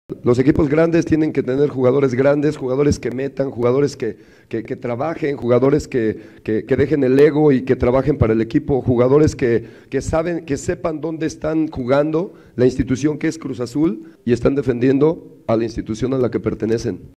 "Necesitamos jugadores sin egos" | Raúl Gutiérrez | D.T. Club Cruz Azul
Desde su llegada tras el 7-0 que le propinó el América, el Cruz Azul con Raúl Gutiérrez está cerca del repechaje y pelear por la liguilla.